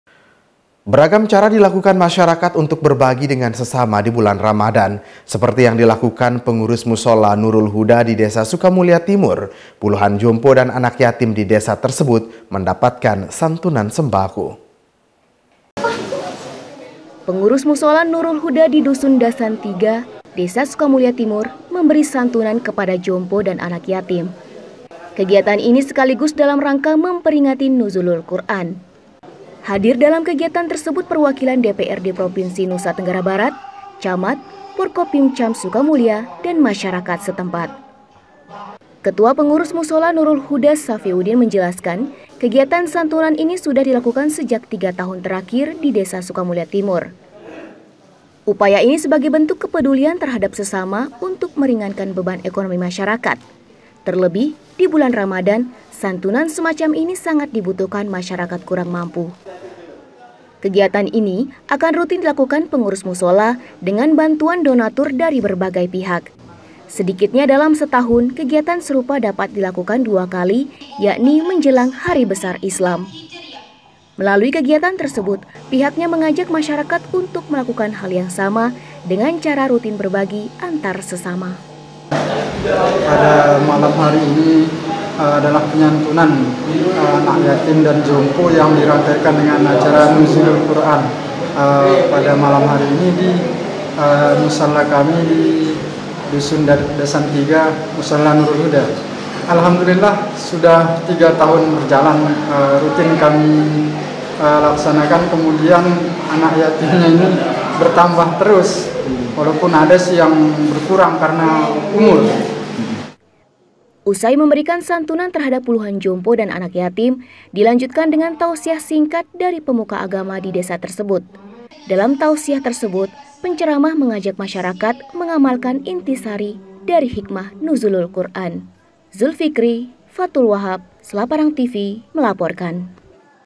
PENGURUS MUSHOLA NURUL HUDA SUKAMULIA TIMUR SANTUNI JOMPO DAN ANAK YATIM
Beragam Cara Dilakukan Masyarakat Untuk Berbagi Dengan Sesama Di Bulan Ramadhan. Seperti Yang Dilakukan Pengurus Mushola Nurul Huda Di Desa Sukamulia Timur, Puluhan Jompo Dan Anak Yatim Di Desa Tersebut Mendapatkan Santunan Sembako.

Berita ini menjadi salah satu yang kami turunkan dalam program berita Jendela Selaparang edisi Senin, 17 April 2023 disamping berbagai kabar menarik lainnya.